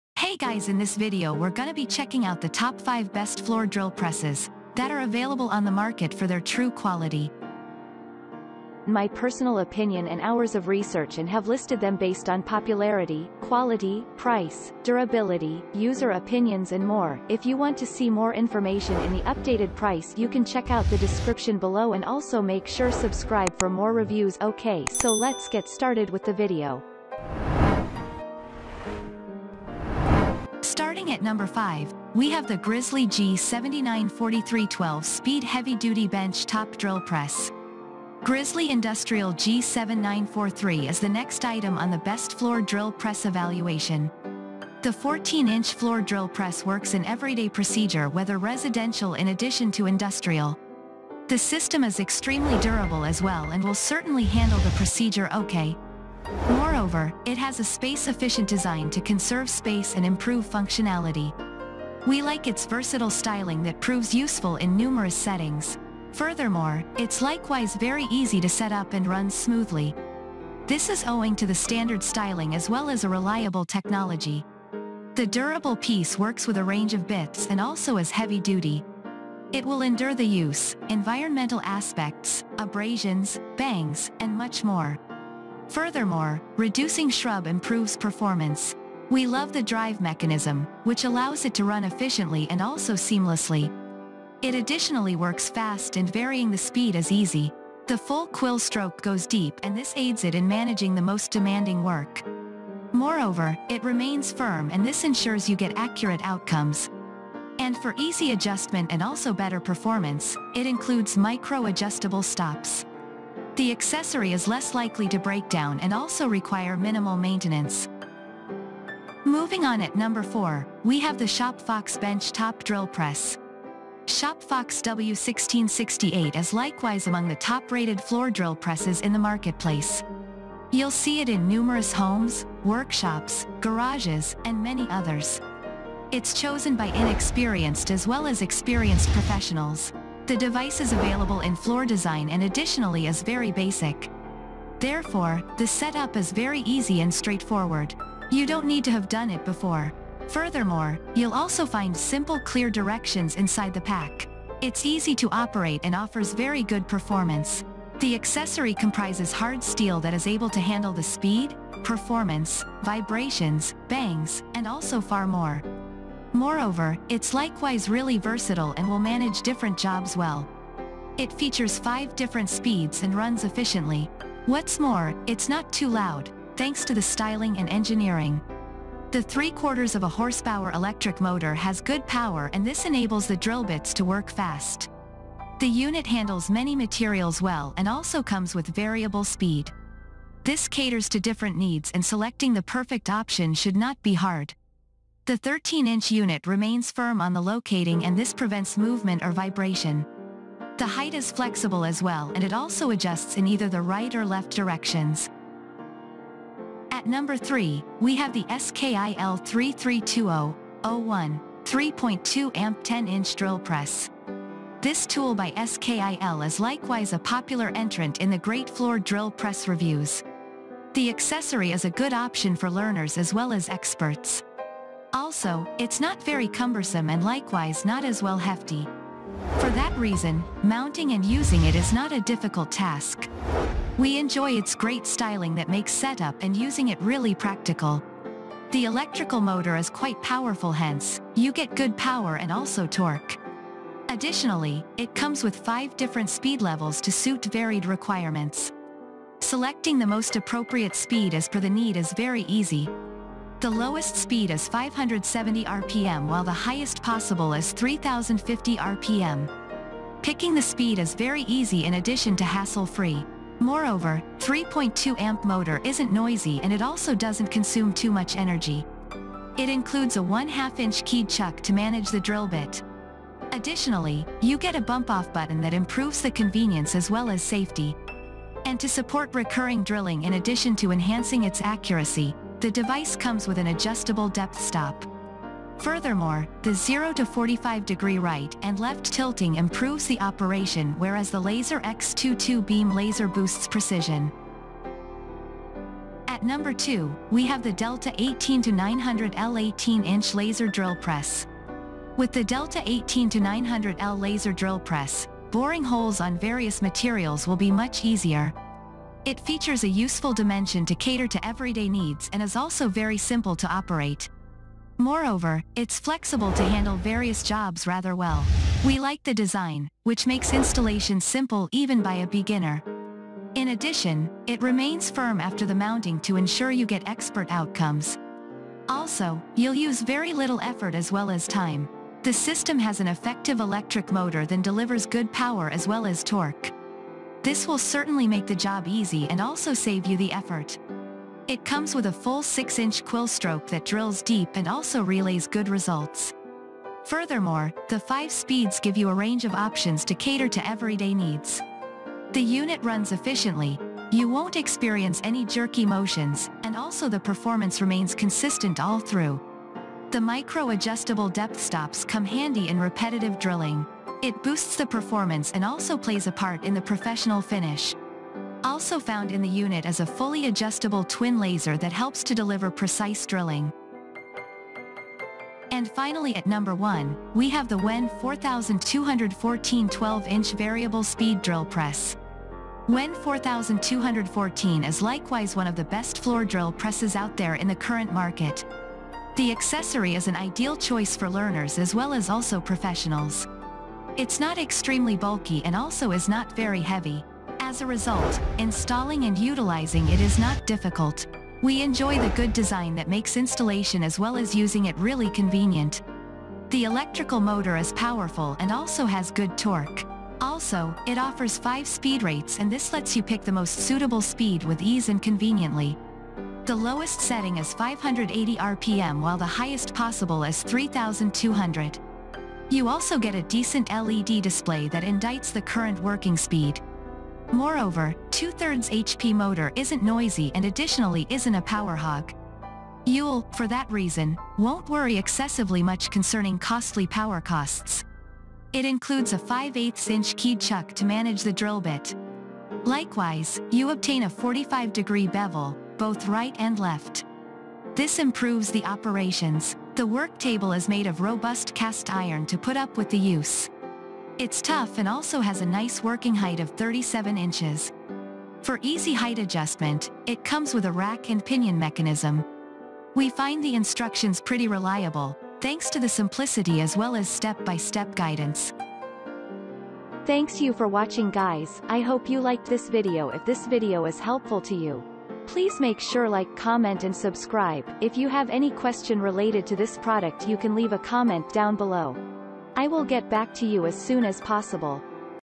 ▶️Top 5 Best Floor Drill Presses Review 2023
▶️ 5.Grizzly G7943 12 Speed Heavy-Duty Bench-Top Drill Press

▶️ 4.Shop Fox Bench-Top Drill Press

▶️ 3.SKIL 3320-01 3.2 Amp 10-Inch Drill Press

▶️ 2. Delta 18-900L 18-Inch Laser Drill Press

▶️ 1.WEN 4214 12-Inch Variable Speed Drill Press